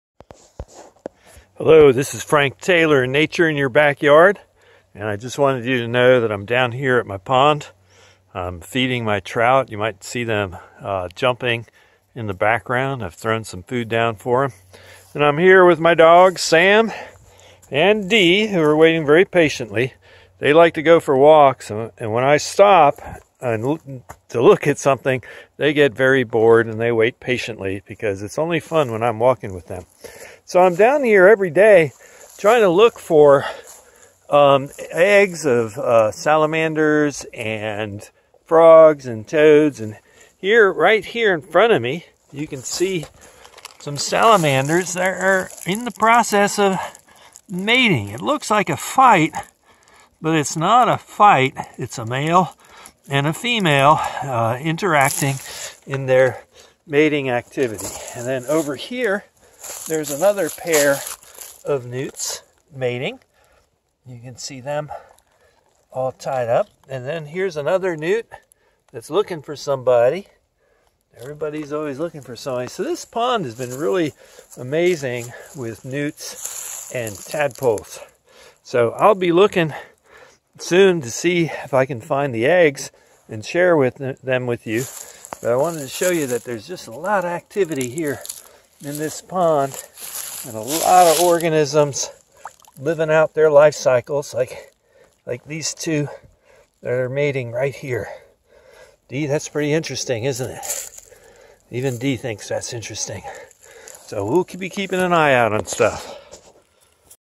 Eastern Newt Breeding April 2020
It is early April so each day I visit this pond in Floyd County Virginia to see what has become active and to look to see if any amphibians have laid eggs. On this day, Eastern Newts, are actively mating and seeking mates. Notes about mating behaviors and egg laying are made.